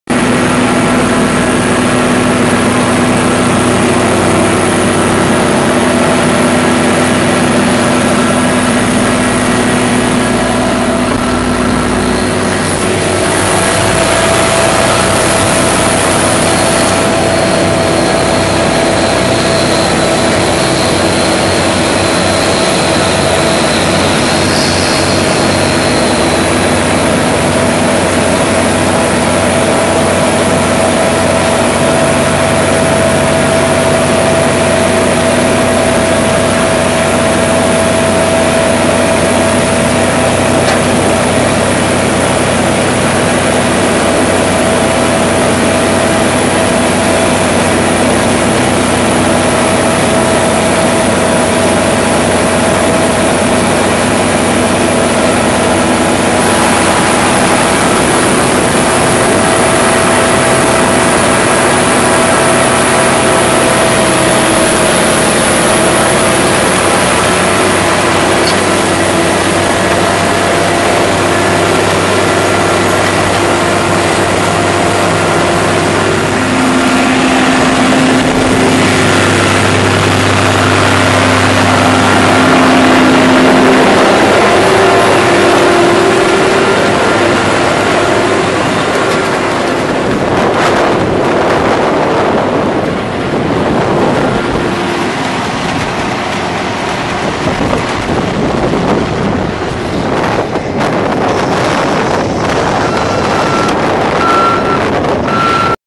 Aardappelen rooien met een 2 rijer en 4 rijer 26-09-12
Op dit perceel staan de Innovator aardappelen. 1 loonwerker rooide 26 september op een perceel Innovator, die gelijk af land weg zijn gegaan. De andere loonwerker rooide op een perceel Agria, die ingeschuurd is.